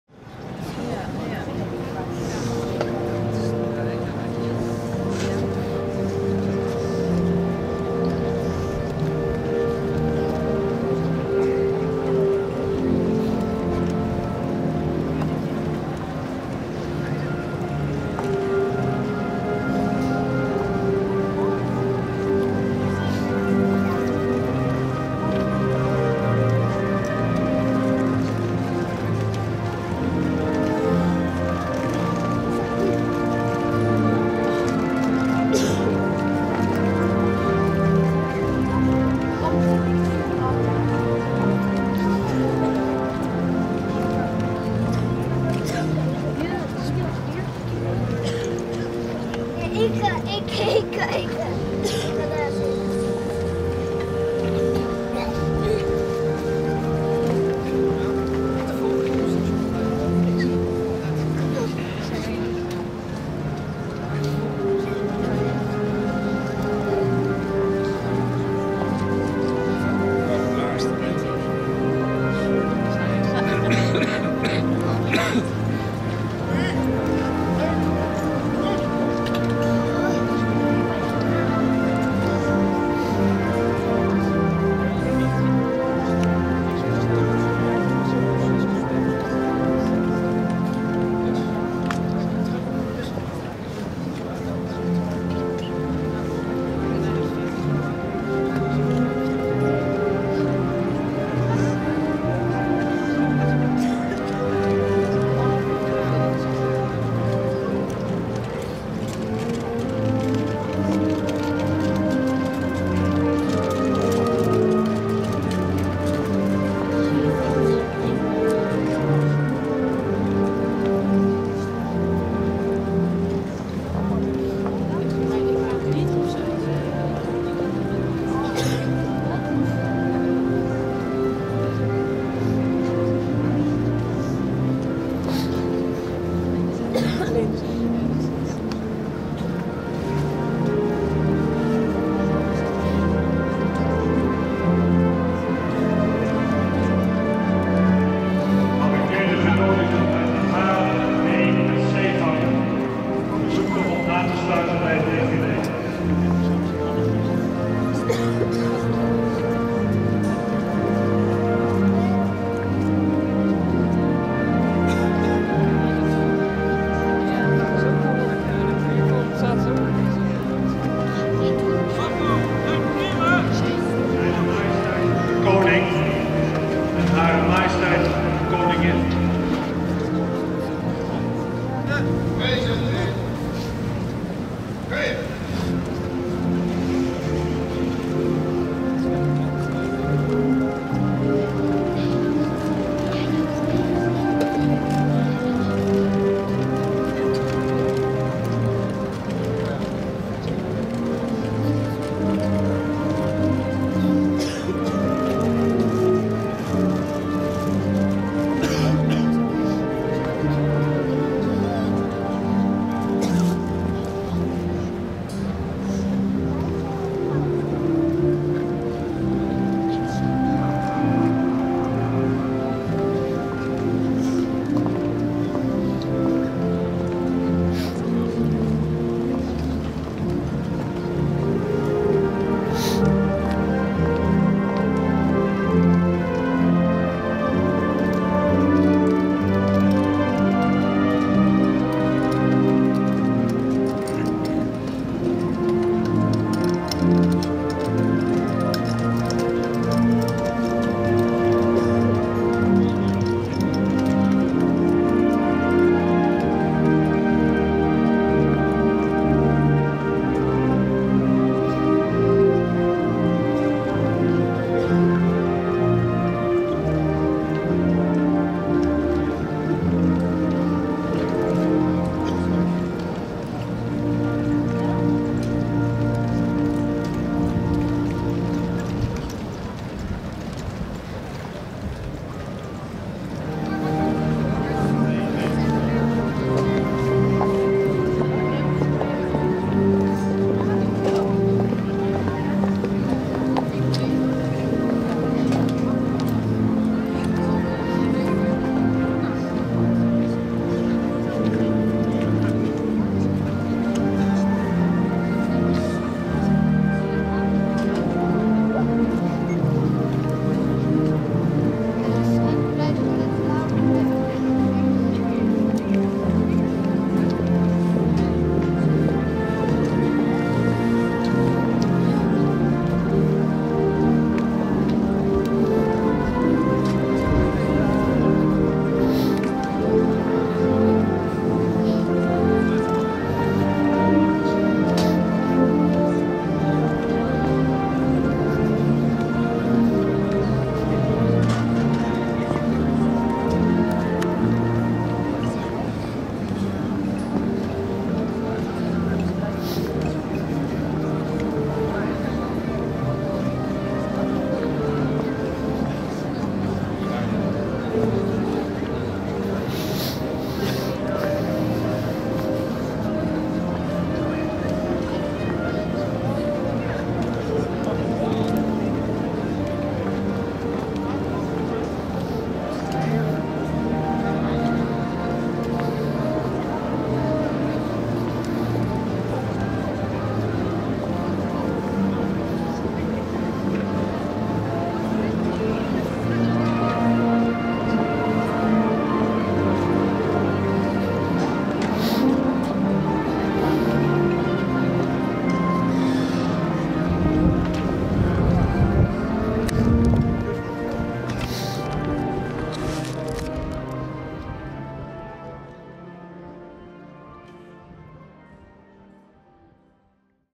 Remembrance Day Amsterdam Dam square, kranslegging op de Dam 4 mei 2018 (Thomas Milo )
Amsterdam, May 4. The Guard of Honour consisting of all branches of the Armed Forces and their veterans on Dam Square, Amsterdam, where the King placed the first wreath to commemorate the fallen in the Second World War and in the ensuing armed conflicts and peace keeping operations.